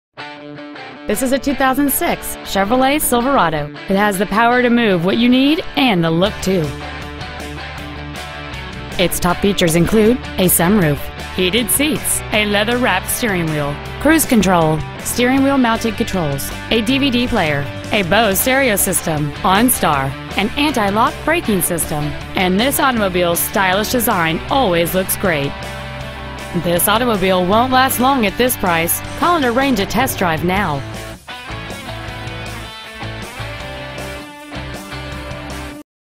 Used 2006 Chevrolet Silverado Auburn WA
Year: 2006
 Make: Chevrolet
 Model: Silverado
 Engine: 6.6L TURBO DIESEL
 Trans.: Automatic
 Exterior: Pewter
 Miles: 90,521
 Interior: Gray